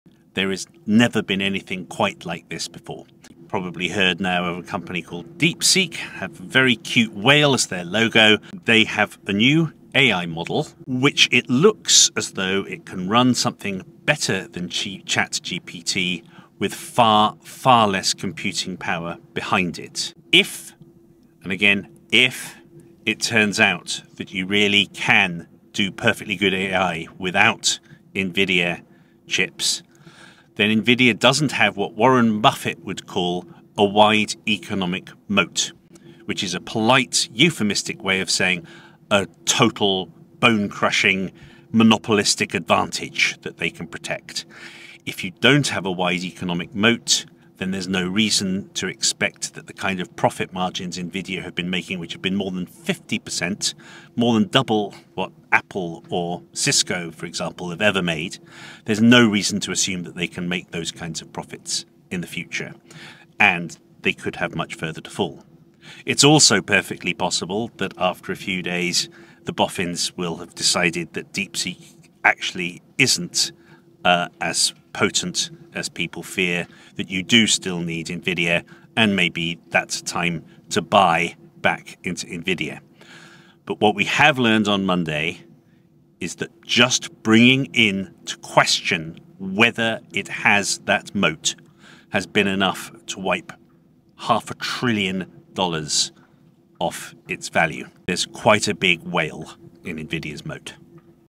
Has Deepseek shown that Nvidia doesn’t have an “economic moat” it needs?
The chipmaker that dominated the AI landscape (and stock market) as we knew it has now been deeply disrupted, says Bloomberg Opinion columnist John Authers. Has DeepSeek shown that Nvidia doesn’t have an “economic moat” after all?